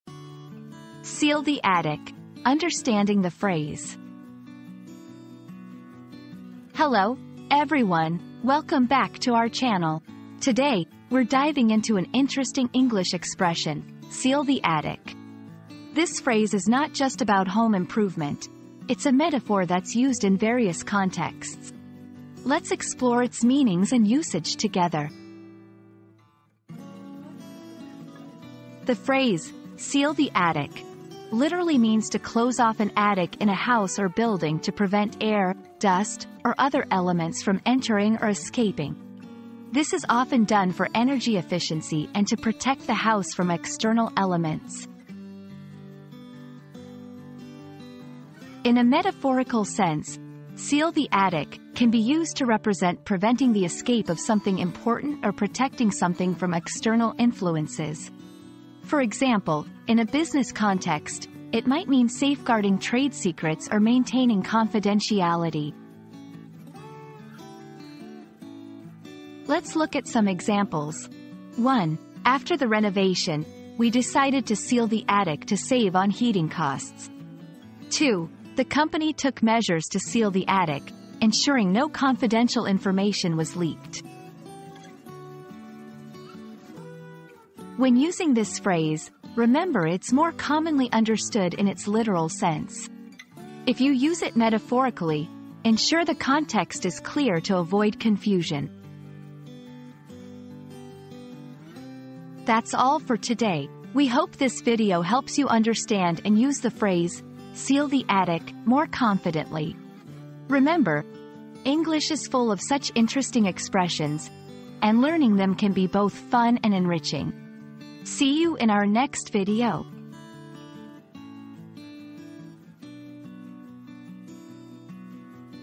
Seal the Attic: Understanding the Phrase
00:00 • Introduction - Seal the Attic: Understanding the Phrase
00:28 • Literal Meaning
00:48 • Metaphorical Usage
01:08 • Examples in Sentences
01:27 • Usage Tips